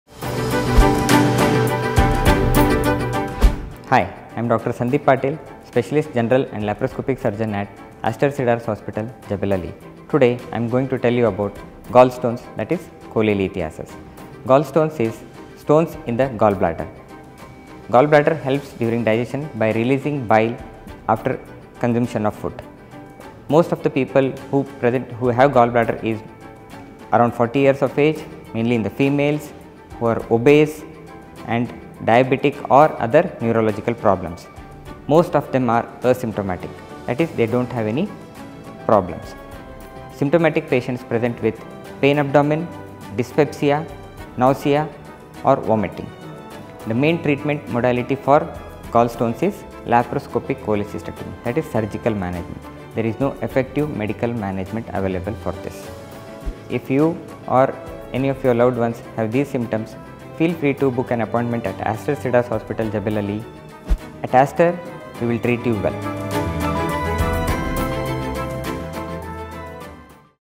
Cholelithiasis or Gallstones - Dr Sandeep Patil | Aster Cedars Hospital and Clinic, Jebel Ali
Meet Dr Sandeep Patil, a Specialist General and Laparoscopic Surgeon at Aster Cedars Hospital and Clinic, Jebel Ali.

Dr Patil dives deep into Cholelithiasis, which are also called Gallstones. These stones are solid particles that form in the gallbladder.

These stones are created when substances like cholesterol harden into solid particles. It can be treated with a surgery called Laparoscopic Cholecystectomy.

To know more about Gallstones, visit Aster Cedars Hospitals and Clinic, Jebel Ali.